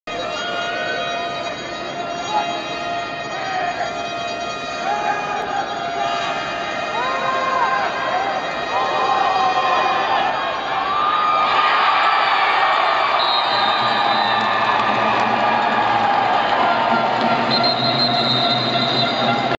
Tshiovo 42 yard gain ends the 1st half
Red River Cup 2014 Gold Medal Game. North Saskatchewan vs. Manitoba Black.